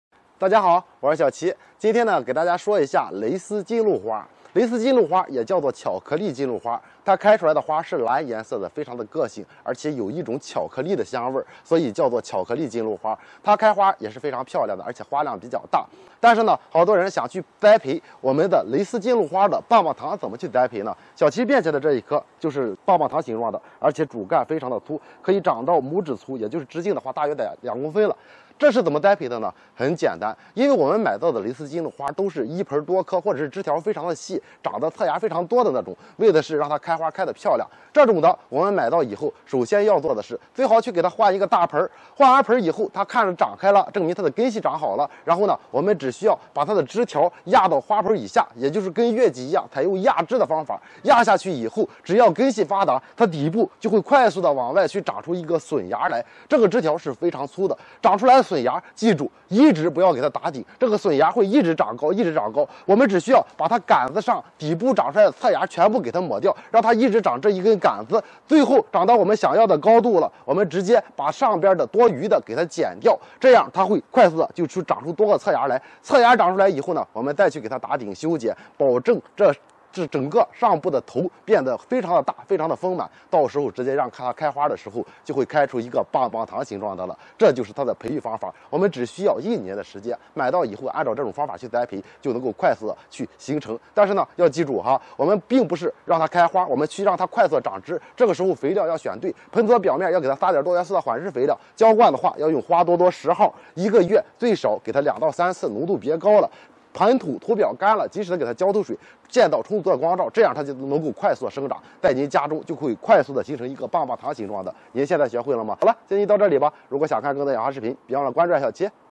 【養養養花花花】蕾絲金露巧克力花香，春季這樣養，輕鬆就開爆！
蕾絲金露花，它的花是藍色的，有淡淡的巧克力香味，也叫巧克力金露花。